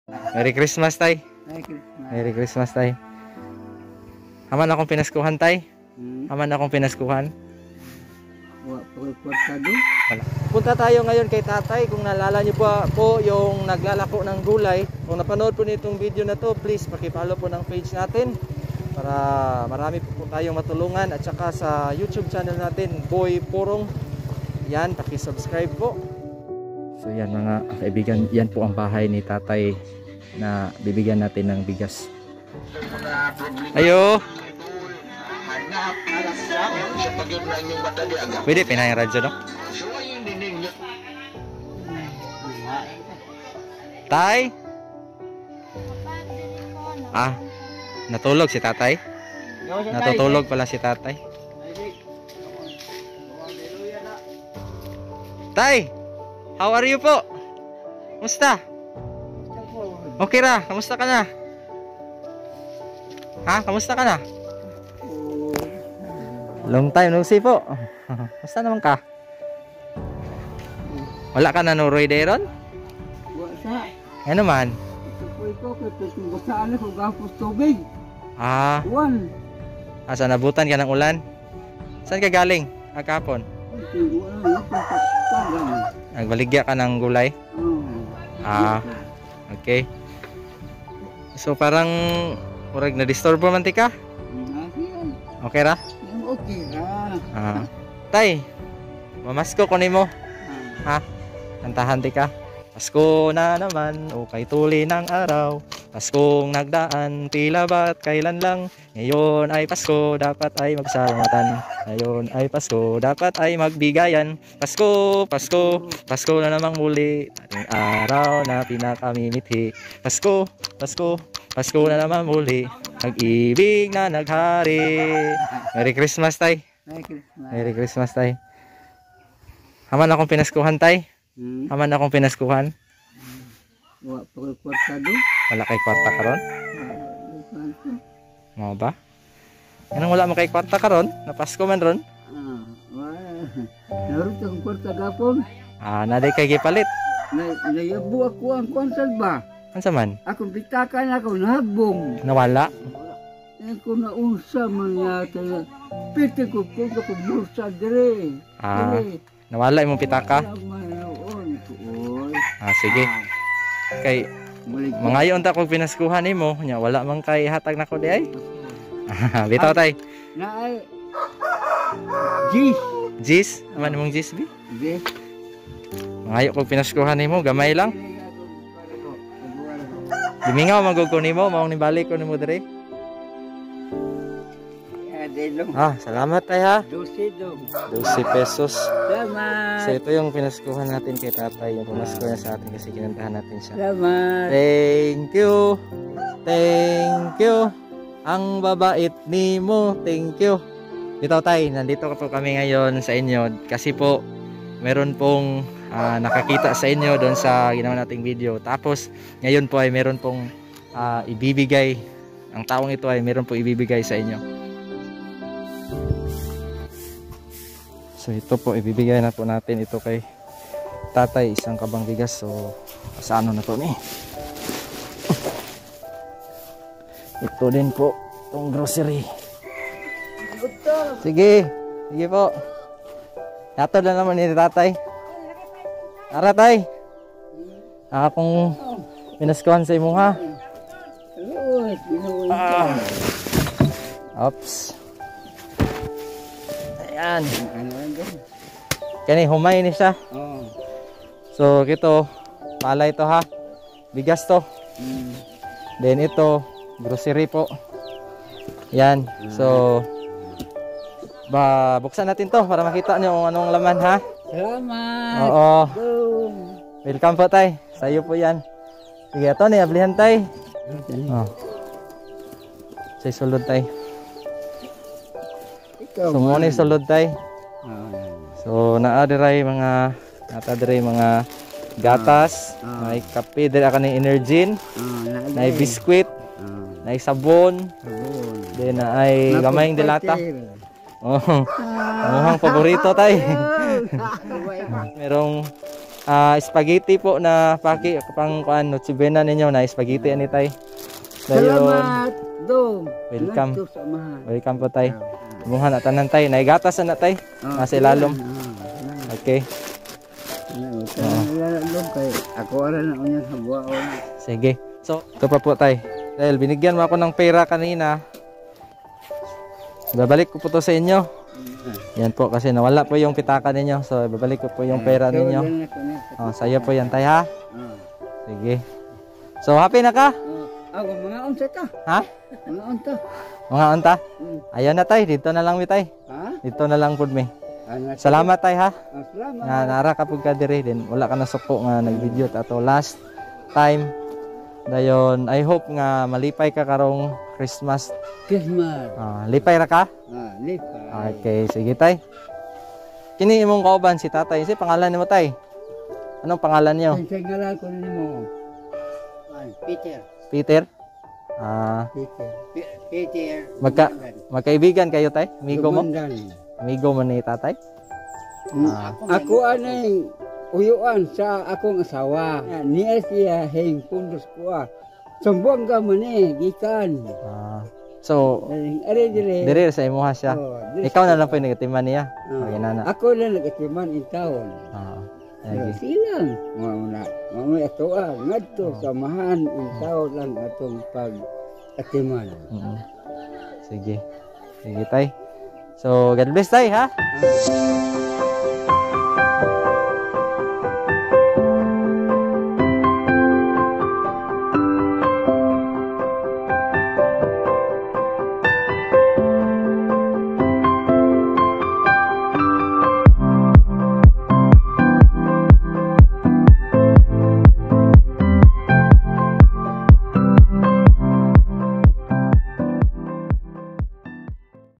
For you tay! Nakakataba ng puso ang kasiyahan ni tatay!
maraming nagsabi hindi daw natulongan si tatay. ito na dinalhan na natin ng regalo. merry christmas tay